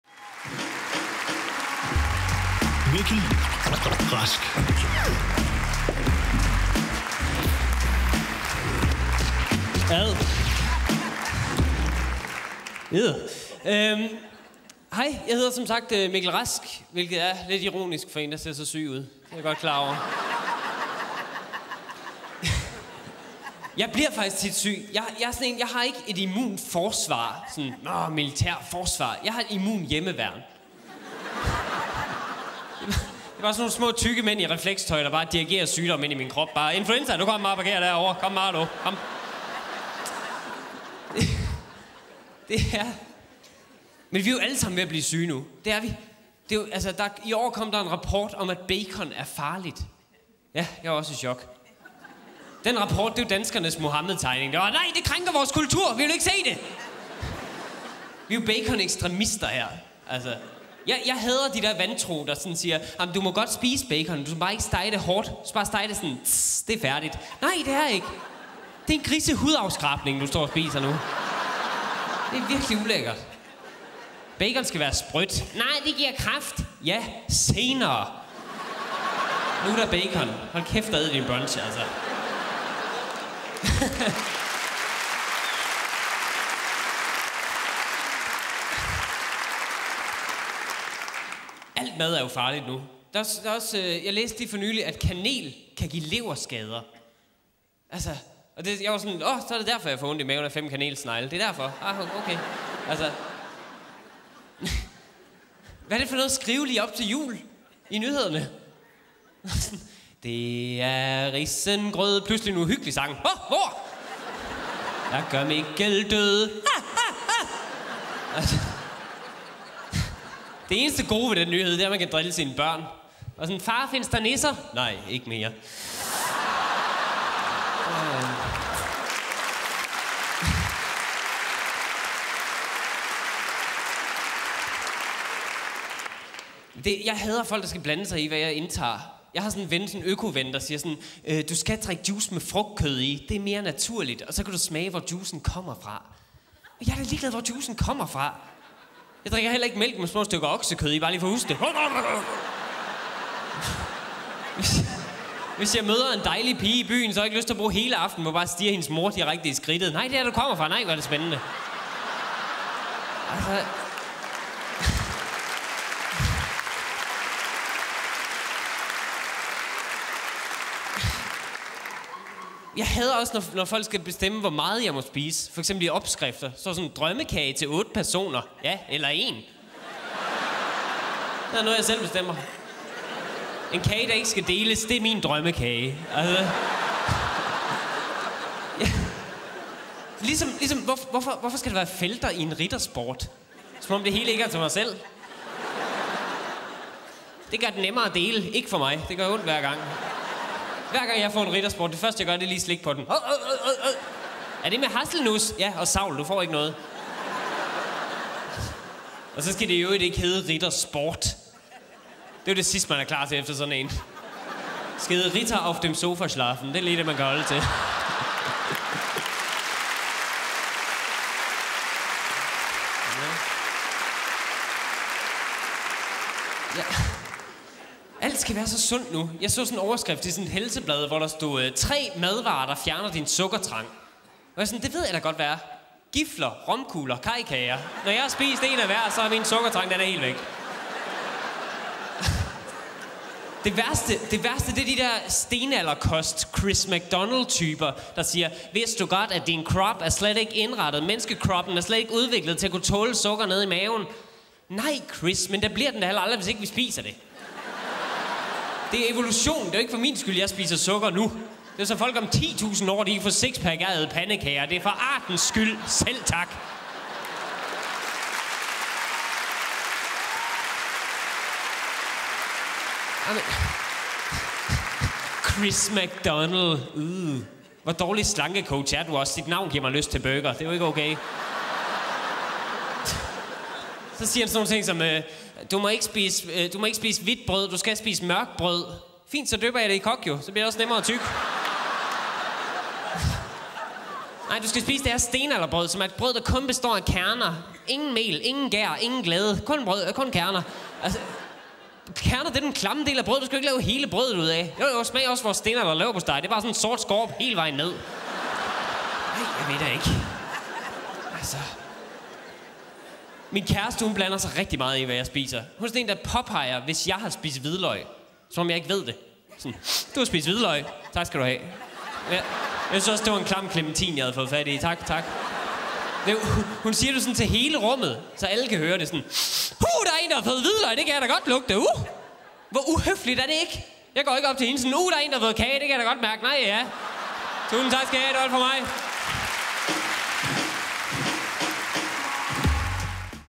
Mikkel Rask - Comedy Aid 2015
Mikkel Rask laver stand-up til Comedy Aid 2015 i Falconer Salen.